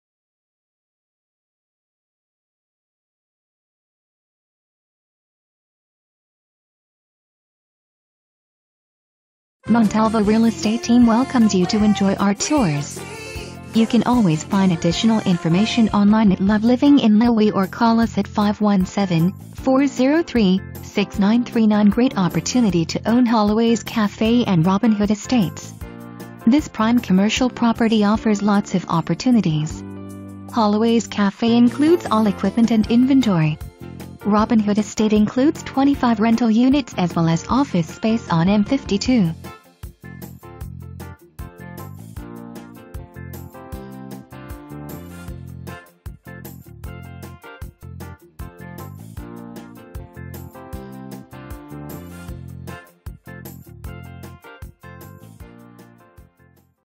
3180 N. Adrian Hwy.
Great Opportunity to own Holloway's Cafe and Robin Hood Estates. This prime commercial property offers lots of opportunities. Holloway's Cafe includes all equipment and inventory. Robin Hood Estate includes 25 rental units as well as office space on M-52.

Presented By:
Janis Montalvo, Living In Lenawee